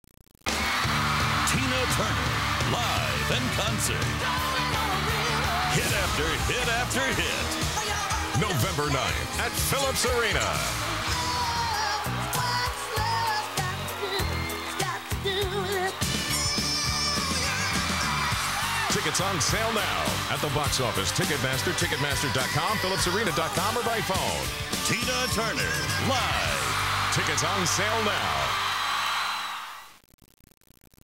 AMWAY GLOBAL Presents TINA TURNER LIVE
Tina Turner Live at Philips Arena on November 9, 2008